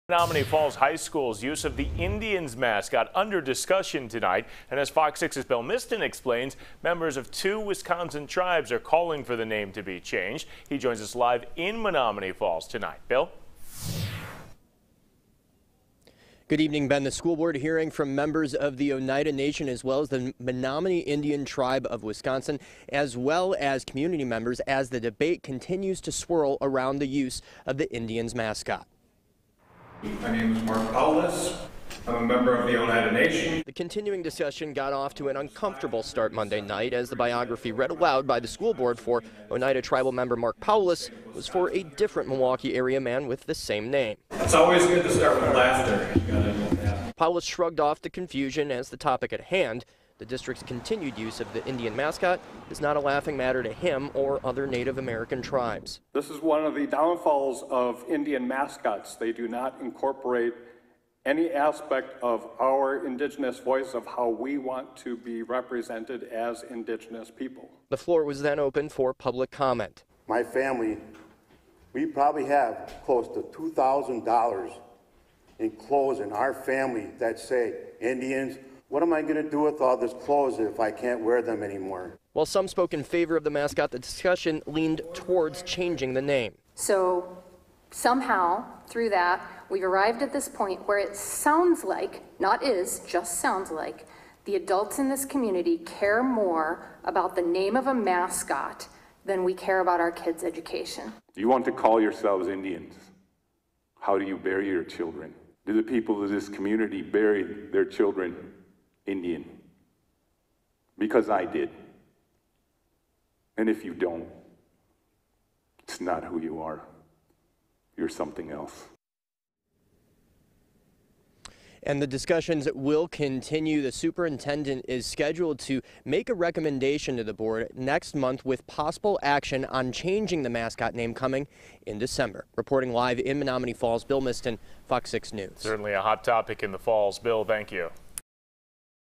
Menomonee Falls` `Indians` mascot debated during school board meeting
Menomonee Falls High School's use of the "Indians" mascot discussed during a school board meeting Monday, Oct. 28 -- with two tribes calling for the name to be changed.